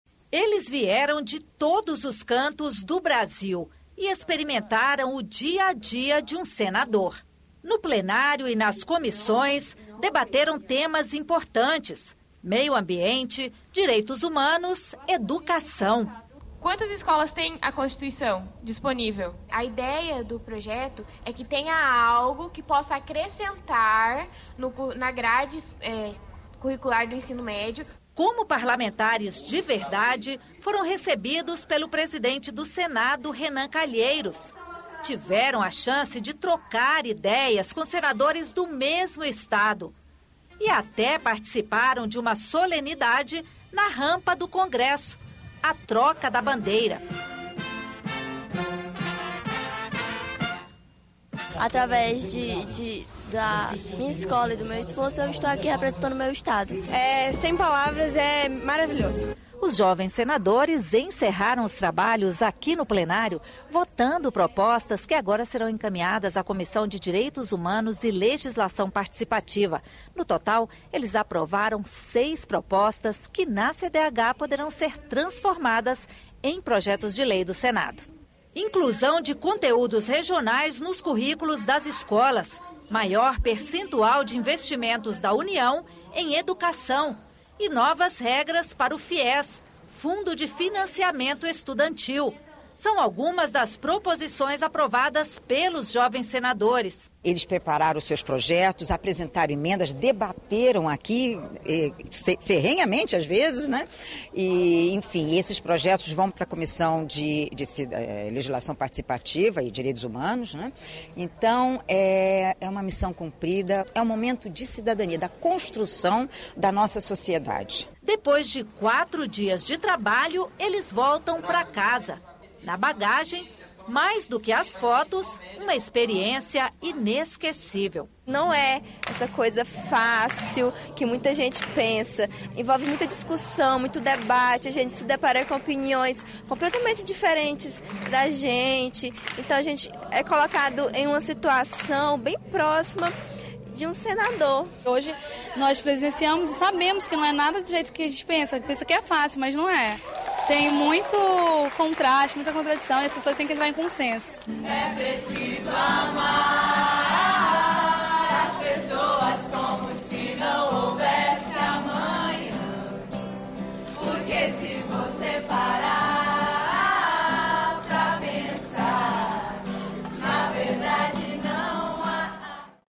'Jovens senadores' encerram 'mandato' de quatro dias
Realizado anualmente, o programa Jovem Senador seleciona alunos das escolas públicas de ensino médio por meio de um concurso de redação. Os vencedores de cada unidade da federação vieram a Brasília, debateram ideias no Senado e encaminharam seis sugestões de projetos de lei que serão analisadas pela Comissão de Direitos Humanos.